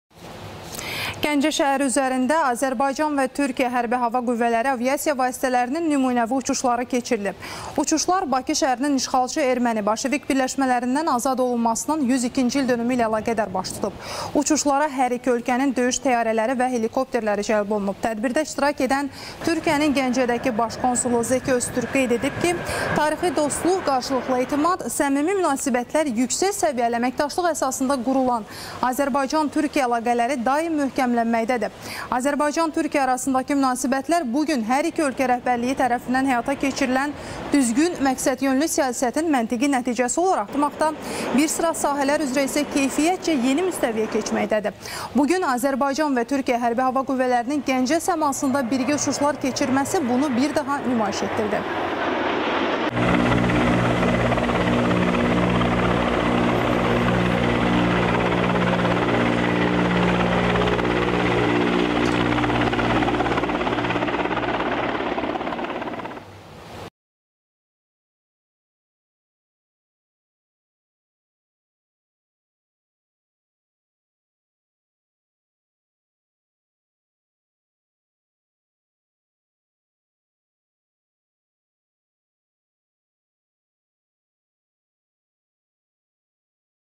Azərbaycan və Türk şahinləri Gəncə səmasında
Gəncə şəhəri üzərində Azərbaycan və Türkiyə Hərbi Hava Qüvvələri aviasiya vasitələrinin nümunəvi uçuşları keçirilib. Uçuşlar  Bakı şəhərinin işğalçı erməni-bolşevik birləşmələrindən azad olunmasının 102-ci ildönümü ilə əlaqədar baş tutub. Uçuşlara hər iki ölkənin döyüş təyyarələri və helikopterləri cəlb olunub. Tədbirdə iştirak edən Türkiyənin Gəncədəki Baş konsulu Zeki Öztürk qeyd edib ki, tarixi dostluq, qarşılıqlı etimad, səmimi münasibətlər, yüksək səviyyəli əməkdaşlıq əsasında qurulan Azərbaycan-Türkiyə əlaqələri daim möhkəmlənməkdədir. Azərbaycan-Türkiyə arasındakı münasibətlər bu gün hər iki ölkə rəhbərliyi tərəfindən həyata keçirilən düzgün, məqsədyönlü siyasətin məntiqi nəticəsi olaraq daha yüksək səviyyəyə çatmaqda, bir sıra sahələr üzrə isə keyfiyyətcə yeni müstəviyə keçməkdədir. Bu gün, Azərbaycan və Türkiyə Hərbi Hava Qüvvələrinin Gəncə səmasında birgə uçuşlar keçirməsi bunu bir daha nümayiş etdirdi.